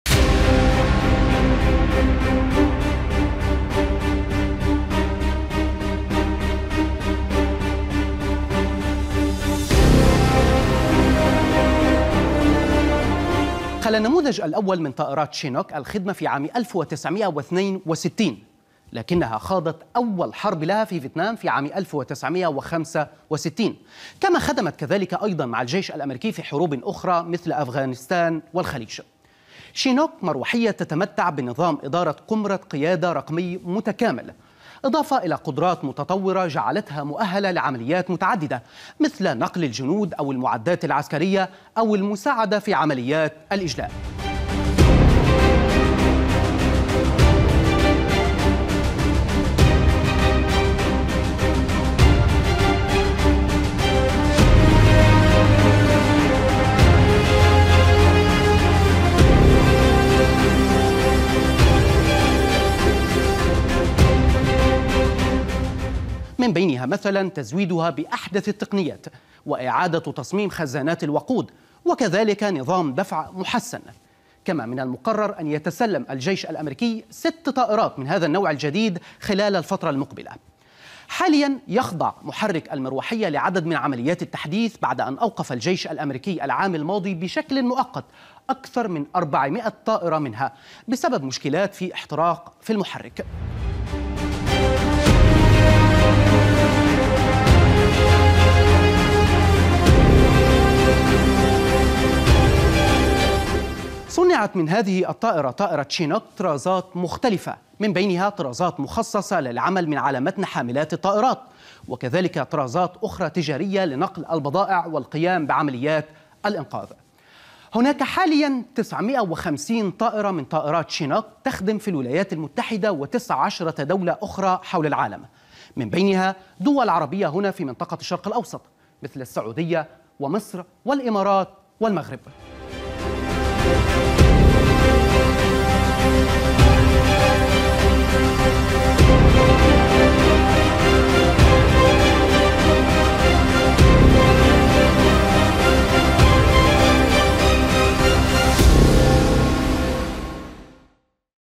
خاضت عدة حروب، ومؤهلة لعمليات نقل الجنود والمعدات العسكرية وعمليات الإجلاء، هي مروحية CH-47 CHINOOK
خاضت عدة حروب، ومؤهلة لعمليات نقل الجنود والمعدات العسكرية وعمليات الإجلاء، هي مروحية CH-47 CHINOOK.. تعرف عليها
لمعرفة أحدث المستجدات.. عبر البث المباشر من هنا ⤵
وبإمكانكم متابعة العربية على منصات التواصل